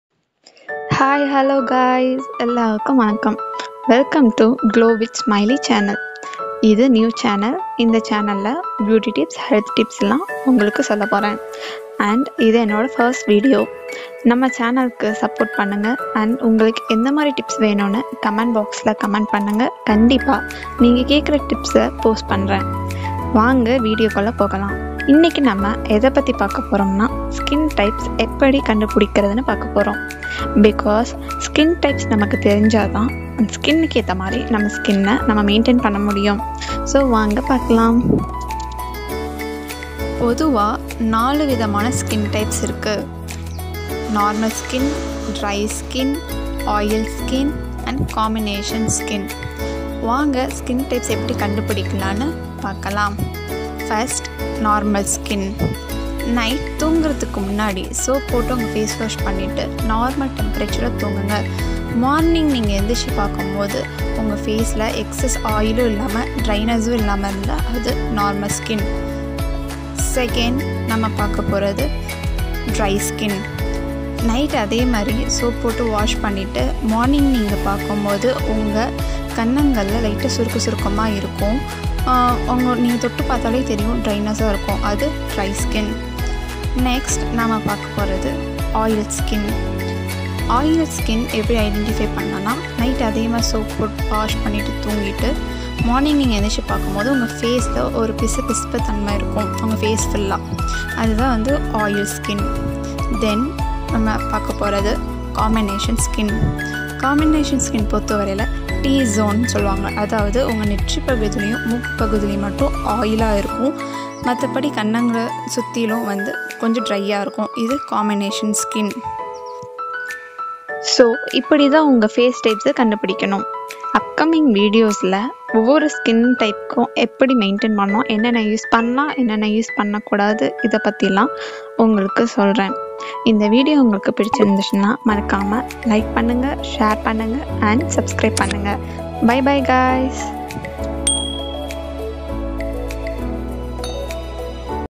Find your Skin Type in just one night /In Tamil/Easy tips to find your skin type!
This is my 1st video 🙈

In this video I give tips to find your skin
Types in just one night 😄

If you need any natural health and beauty tips
Kindly post in comment box, I will post it.